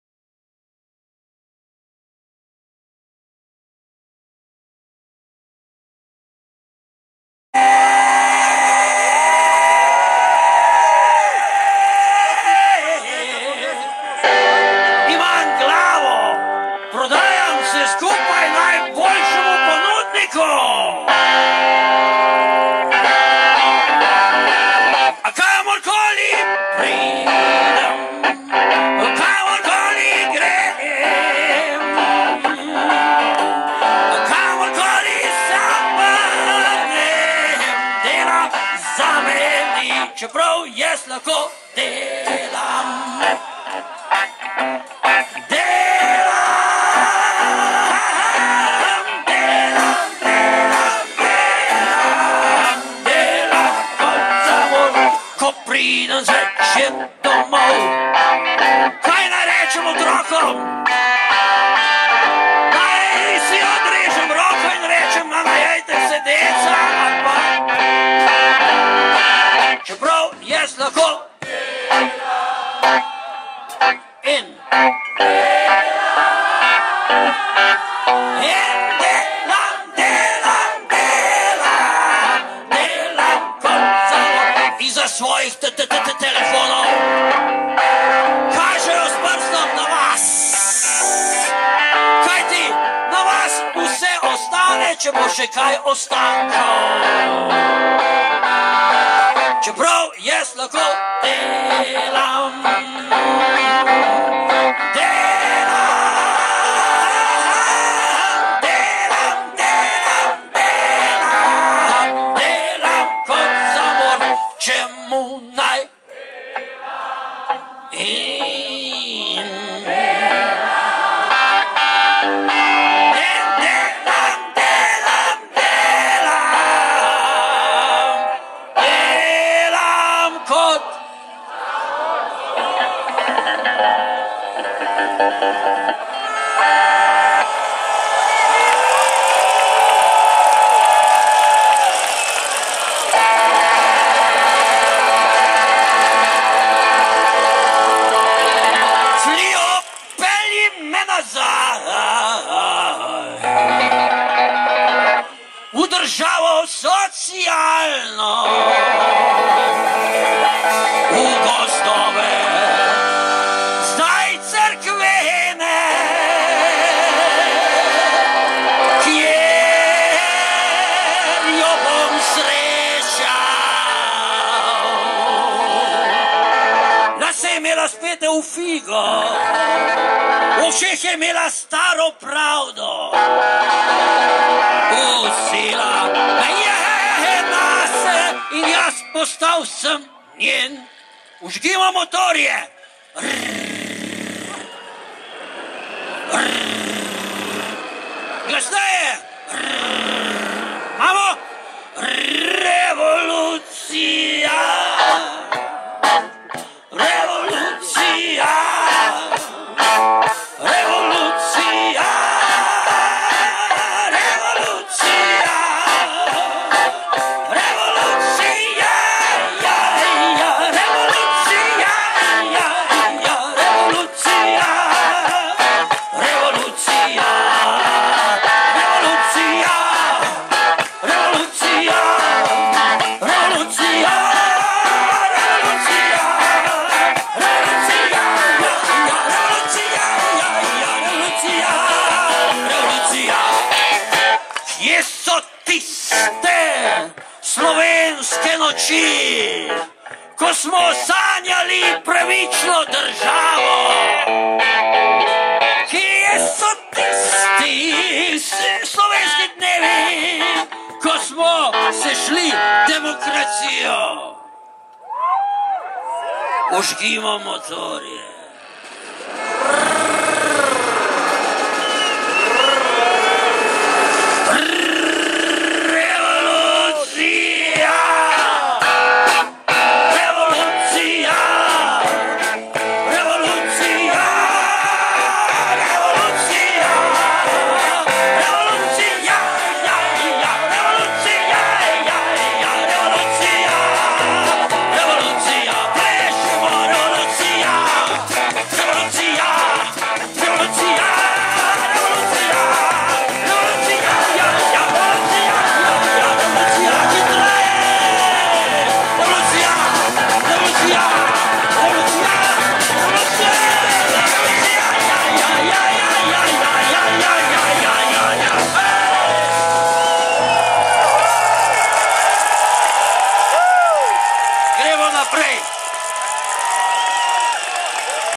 Protesti 11 1 2013 Jani Kovačič
Na protestih proti trenutni politiki v Ljubljani je sodelo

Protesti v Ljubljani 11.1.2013